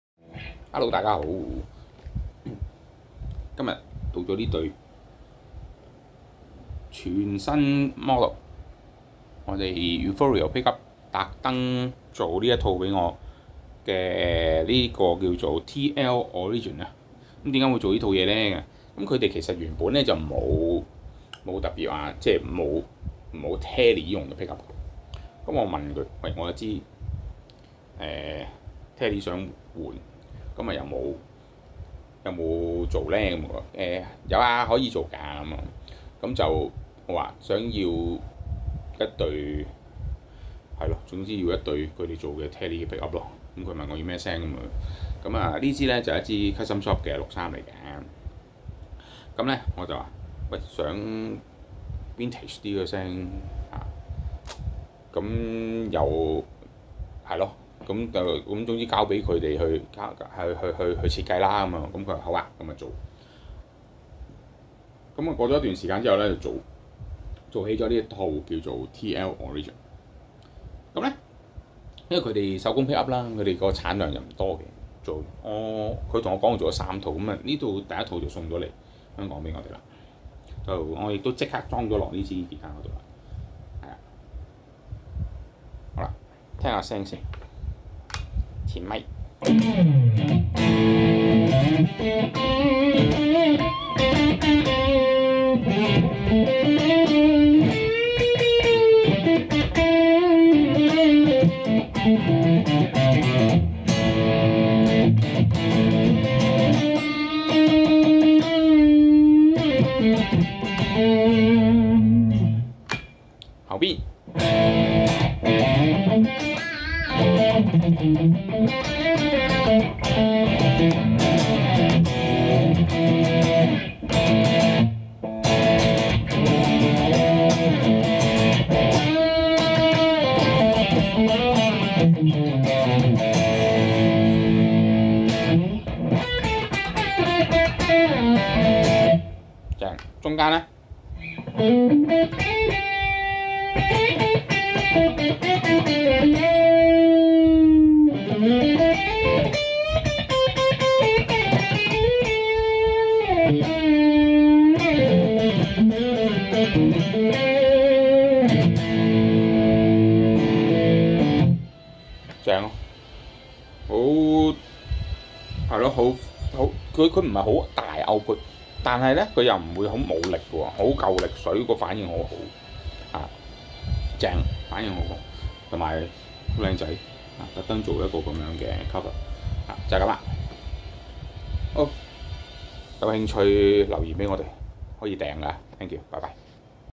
Euphoreal Pickups TL-Origin on Fender Custom 63 Telecaster
Euphoreal 第一套Tele Pickups，就送左黎香港！
有新野當然即裝即試！
聽下呢套咁靚仔既pickups個聲如何先！